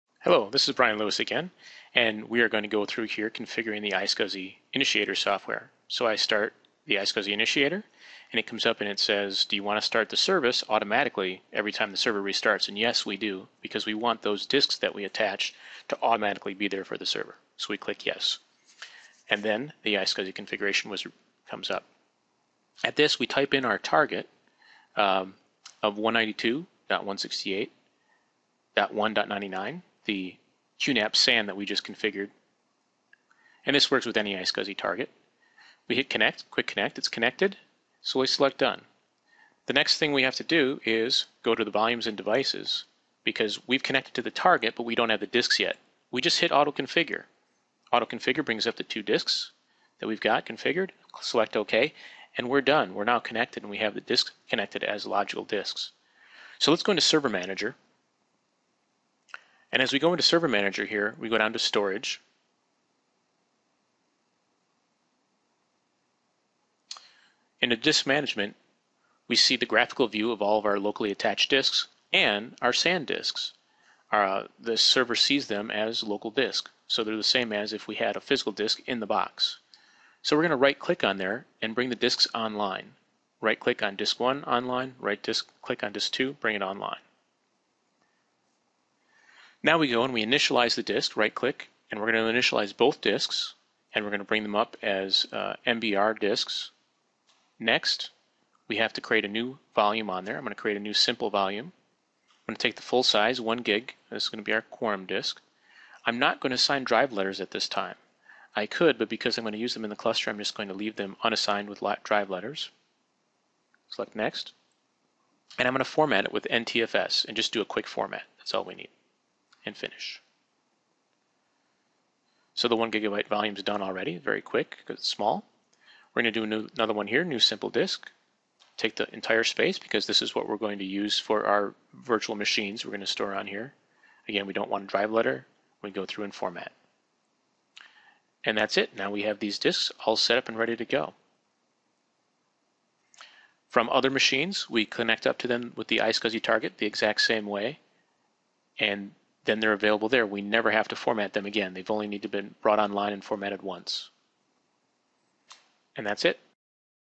Configuring the Windows iSCSI initiator
Configuring the Microsoft Windows iSCSI initiator for use in a Hyper-v 2008 R2 virtualization farm.